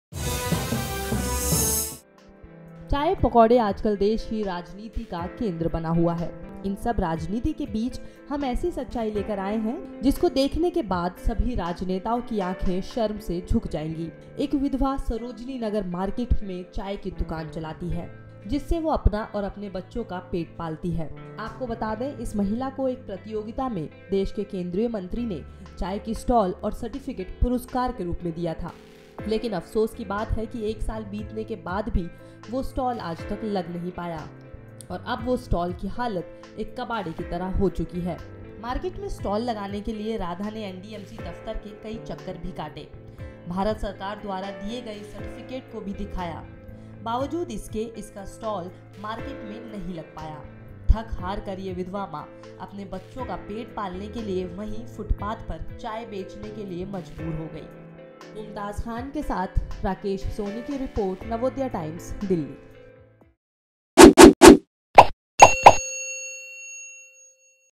पुरस्कार में मिला स्टॉल , लेकिन फुटपाथ पर चाय बेचने के लिए मजबूर
दिल्ली के सरोजनी नगर में फुटपाथ पर चाय  बेच रही इस महिला को मिला था स्टॉल ...लेकिन स्टॉल बन गया  कबाड़ा, देखिए क्यों ?